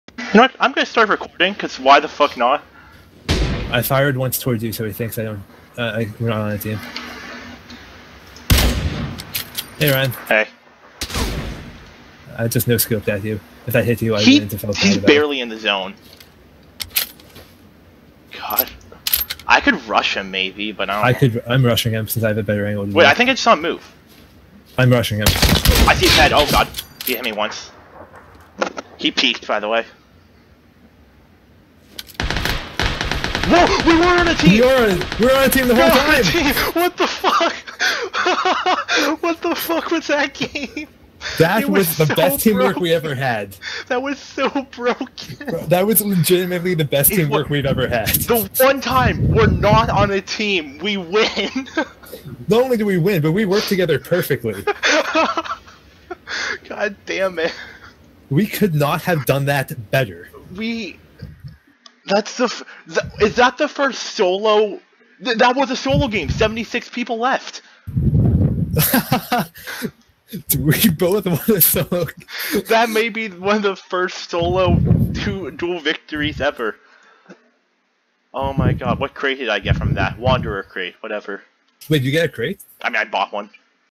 Duo Chicken Dinner in Solos?
This was in the new FPP mode, which at the moment, is SUPER buggy.